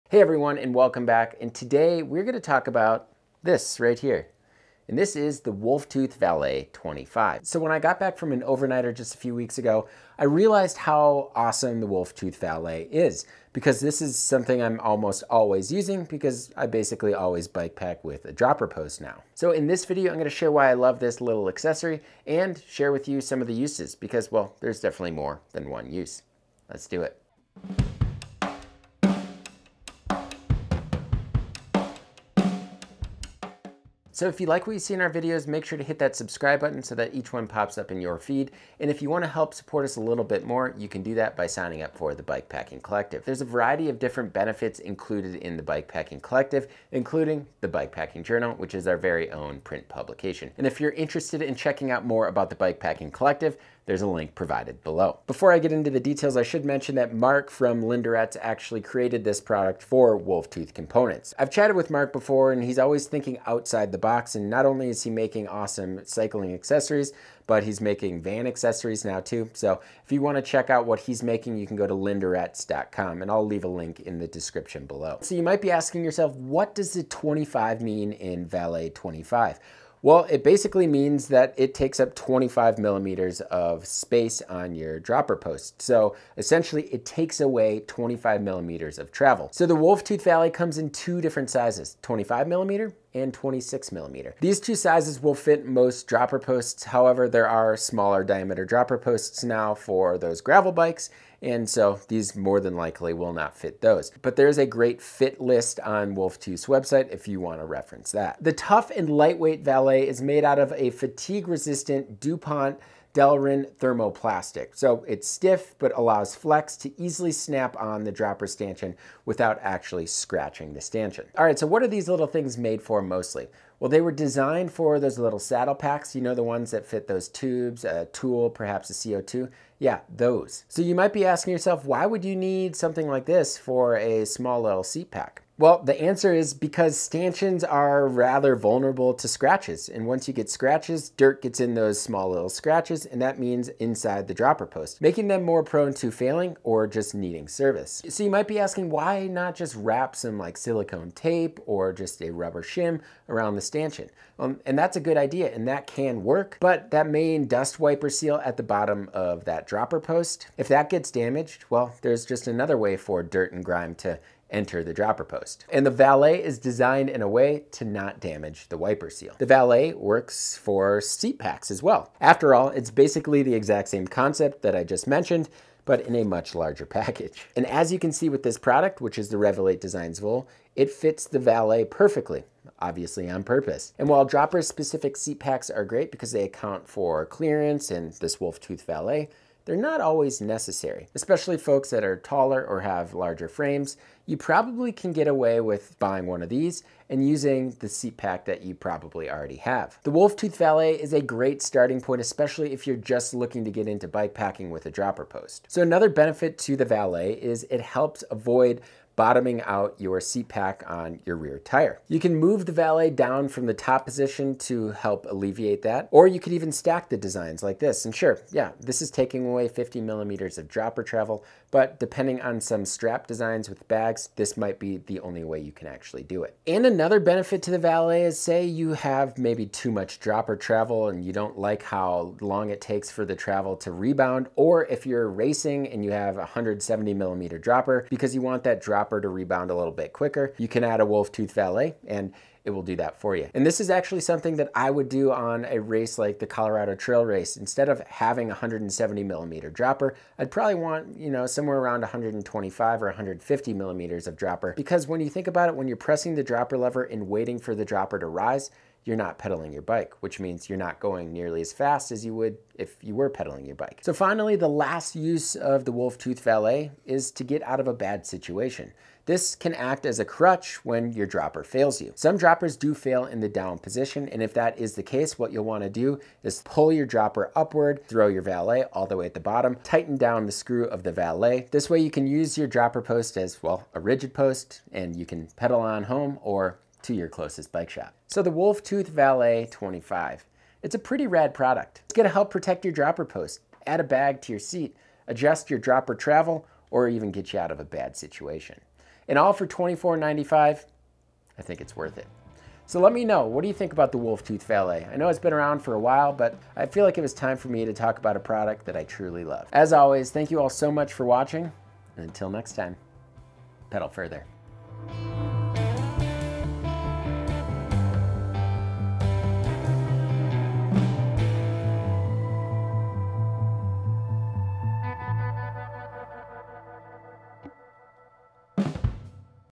The Best Little Bikepacking Accessory?
In this video, Neil shares one of his long time favorite little accessories. And although it's a very small component, it's quite useful and absolutely necessary for bikepacking with a dropper post. Here's why...

Outline:
intro: 0:00
Support and subscribe: 0:33
Product details: 0:55
Valais uses: 2:01
Wrap up: 4:48

Do you enjoy our videos, routes, and articles? Help sustain this resource by joining the Bikepacking Collective